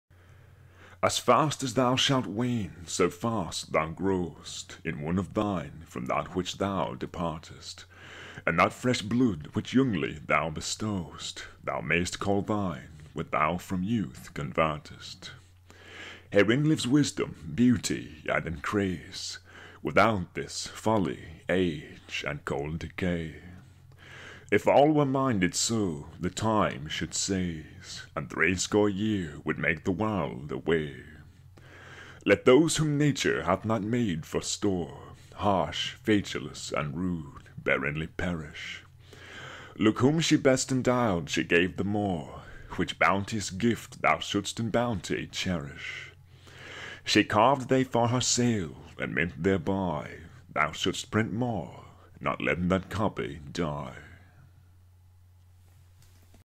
Shakespeare's Sonnet 11, "As fast as thou shalt wane, so fast thou grow'st", read with OP
As fast as thou shalt wane, so fast thou grow'st
In one of thine, from that which thou departest;
And that fresh blood which youngly thou bestow'st,
Thou mayst call thine when thou from youth convertest.
Herein lives wisdom, beauty, and increase;
Without this folly, age, and cold decay:
If all were minded so, the times should cease
And threescore year would make the world away.
Let those whom nature hath not made for store,
Harsh, featureless, and rude, barrenly perish:
Look whom she best endowed, she gave the more;
Which bounteous gift thou shouldst in bounty cherish:
   She carved thee for her seal, and meant thereby,
   Thou shouldst print more, not let that copy die.